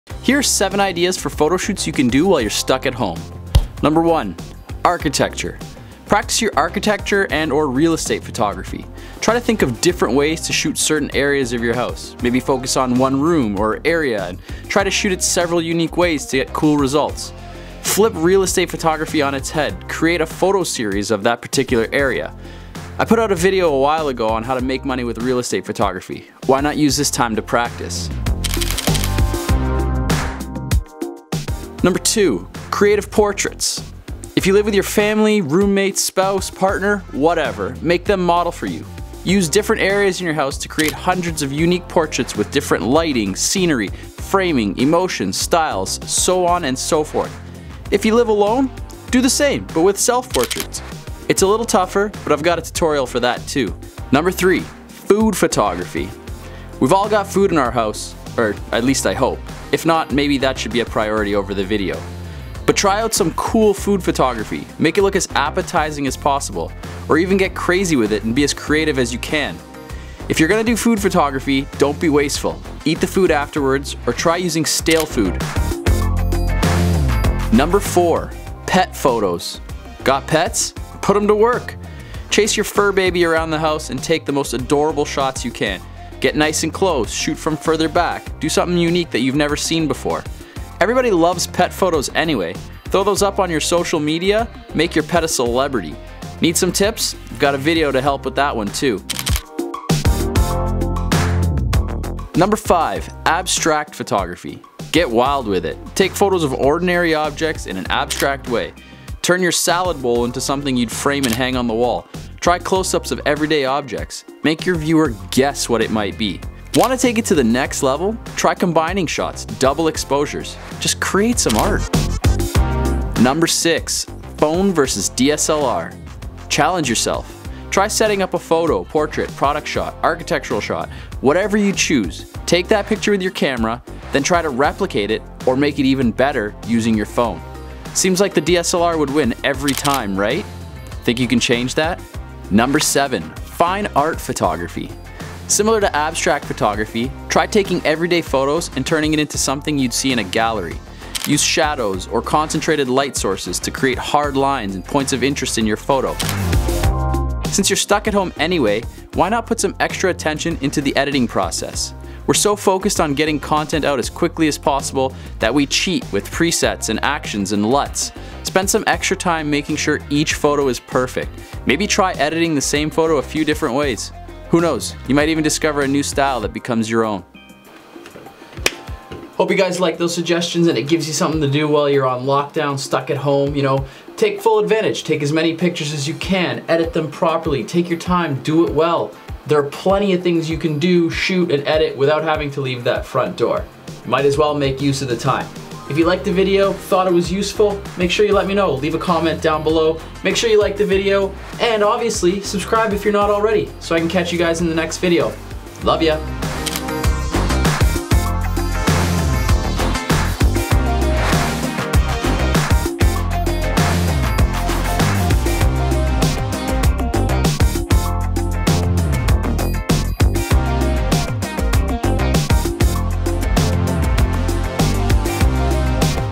7 PHOTOSHOOT IDEAS for when you're STUCK AT HOME
Here are 7 ideas you can execute while you're isolated at home, being safe! Give them a shot, try them all, see what you can produce. I'm sure there are tonnes more to explore! Take this time to perfect your craft, work on those skills, and create something awesome.